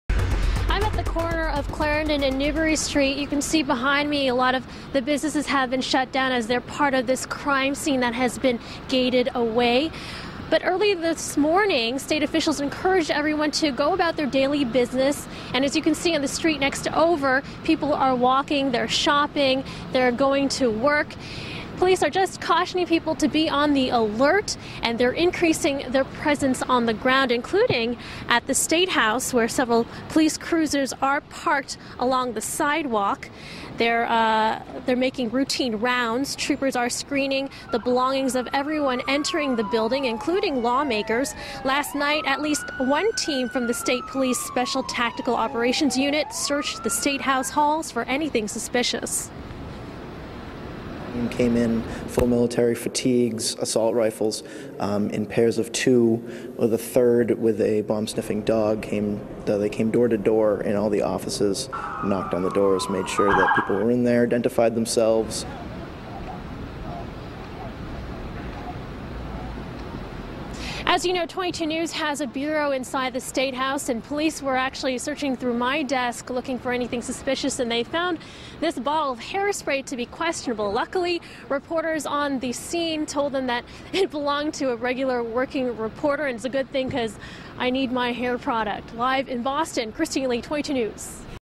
Increased security on Beacon Hill
State police are stepping up security on Beacon Hill.